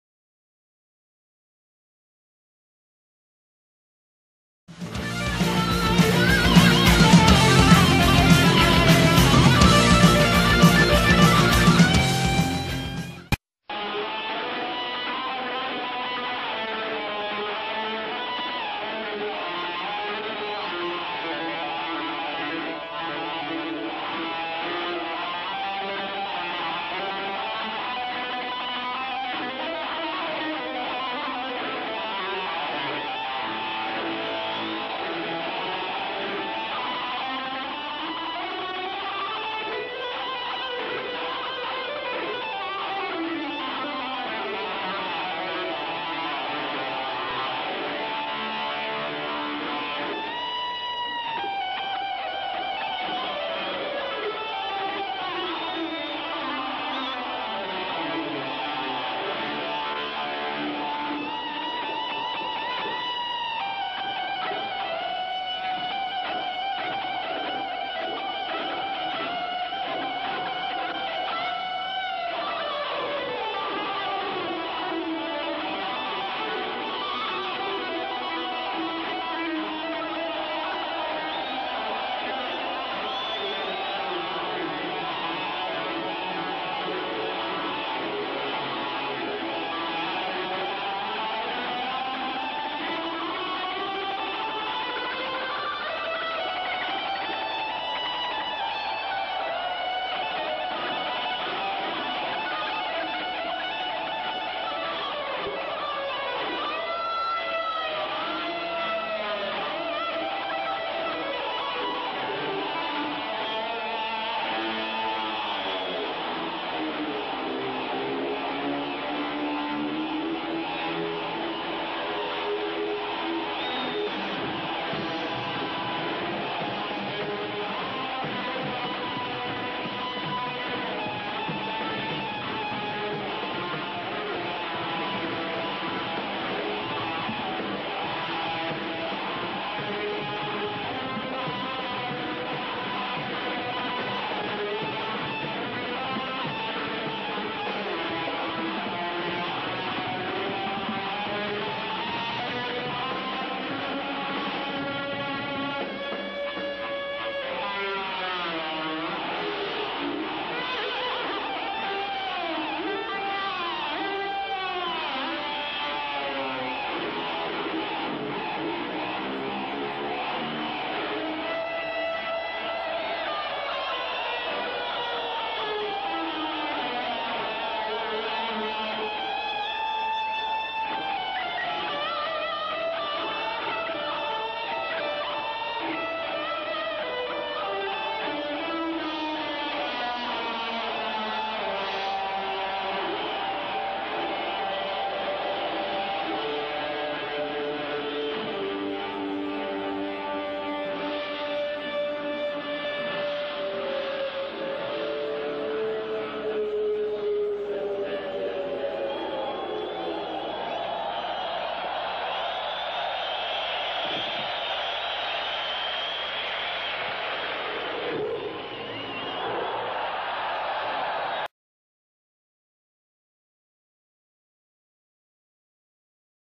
Dave Murray: gran solo de guitarra (1985)
gran solo de Dave Murray en el Rock in Rio de 1985...